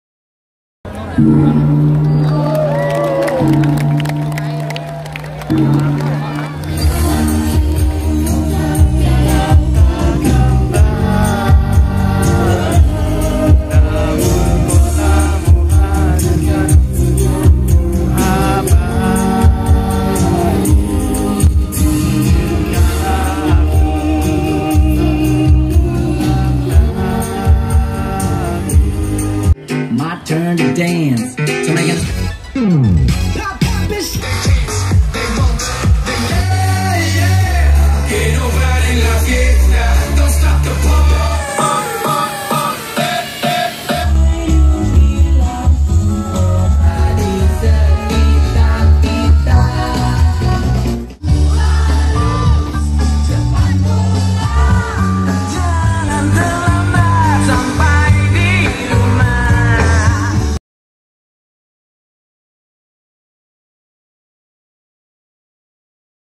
Ulang Tahun Kota Yogyakarta ke 262 kinemaster editing
Beginilah rangkuman puncak acara HUT Jogja #262 yang sangat meriah. Karena terlambat datang jadi tidak bisa menyaksikan kirab dari dekat. Walaupun demikian, masih bisa menikmati dugem bersama.

Nikmati hutkotajogja262 wjnc3 wjnc2018 dalam video beresolusi qhd60fps yang direkam dengan samsunggalaxys8 dan diedit dengan